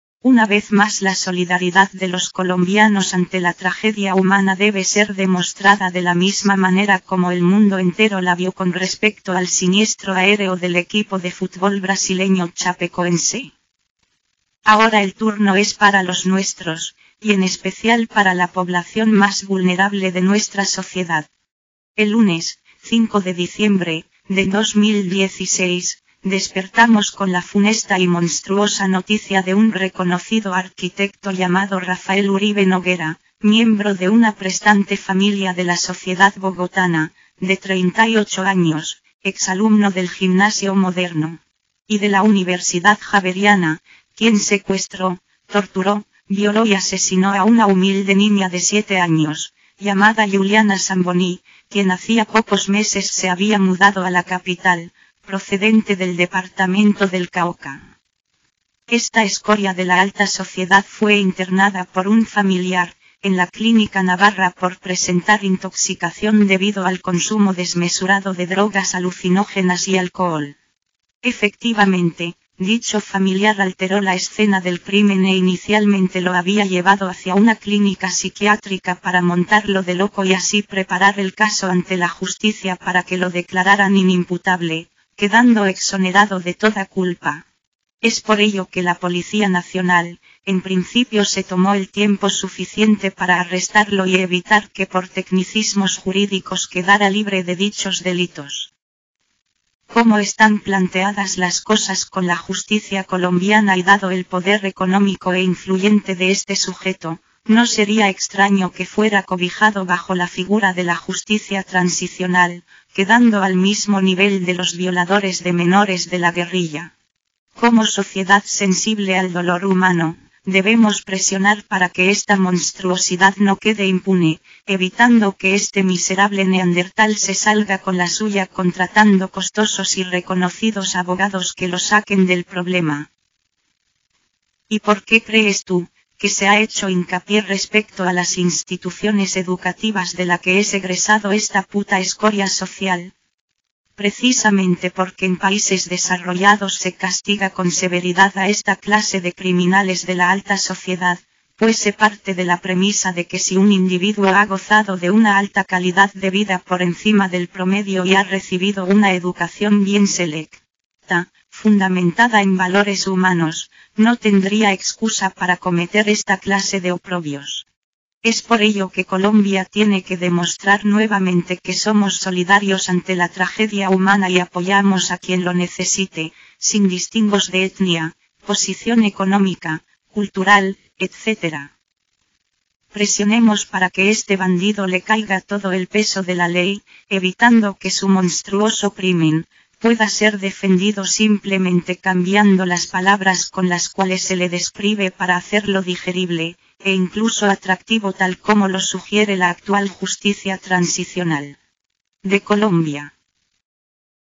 Ni una más: Yuliana
Una vez más la solidaridad de los colombianos ante la tragedia humana debe ser demostrada de la misma manera como el mundo entero la vio con respecto al siniestro aéreo del equipo de fútbol brasileño Chapecoense.

Ahora el turno es para los nuestros, y en especial para la población más vulnerable de nuestra sociedad. El lunes 5 de diciembre de 2016, despertamos con la funesta y monstruosa noticia de un reconocido arquitecto llamado RAFAEL URIBE NOGUERA, miembro de una prestante familia de la sociedad bogotana, de 38 años, ex alumno del Gimnasio Moderno y de la Universidad Javeriana, quién secuestró, torturó, violó y asesinó a una humilde niña de 7 años, llamada Yuliana Samboní, quien hacía pocos meses se había mudado a la capital, procedente del departamento del Cauca.

Esta escoria de la alta sociedad fue internada por un familiar, en la Clínica Navarra por presentar intoxicación debido al consumo desmesurado de drogas alucinógenas y alcohol. Efectivamente, dicho familiar alteró la escena del crimen e inicialmente lo había llevado hacia una clínica siquiátrica para montarlo de loco y así preparar el caso ante la justicia para que lo declararan inimputable, quedando exonerado de toda culpa. Es por ello que la Policía Nacional, en principio se tomó el tiempo suficiente para arrestarlo y evitar que por tecnicismos jurídicos quedara libre de dichos delitos.

Como están planteadas las cosas con la justicia colombiana y dado el poder económico e influyente de este sujeto, no sería extraño que fuera cobijado bajo la figura de la justicia transicional, quedando al mismo nivel de los violadores de menores de la guerrilla. Como sociedad sensible al dolor humano, debemos presionar para que esta monstruosidad no quede impune, evitando que este miserable neandertal se salga con la suya contratando costosos y reconocidos abogados que lo saquen del problema.

¿Y por qué crees tú, que se ha hecho hincapié respecto a las instituciones educativas de la que es egresado esta puta escoria social? Precisamente porque en países desarrollados se castiga con severidad a esta clase de criminales de la alta sociedad, pues se parte de la premisa de que si un individuo ha gozado de una alta calidad de vida por encima del promedio y ha recibido una educación bien selecta, fundamentada en valores humanos, no tendría excusa para cometer esta clase de oprobios. Es por ello que Colombia tiene que demostrar nuevamente que somos solidarios ante la tragedia humana y apoyamos a quien lo necesite, sin distingos de etnia, posición económica, cultural, etcétera.

Presionemos para que este bandido le caiga todo el peso de la ley, evitando que su monstruoso crimen, pueda ser defendido simplemente cambiando las palabras con las cuales se le describe para hacerlo digerible, e incluso atractivo tal como lo sugiere la actual justicia transicional de Colombia.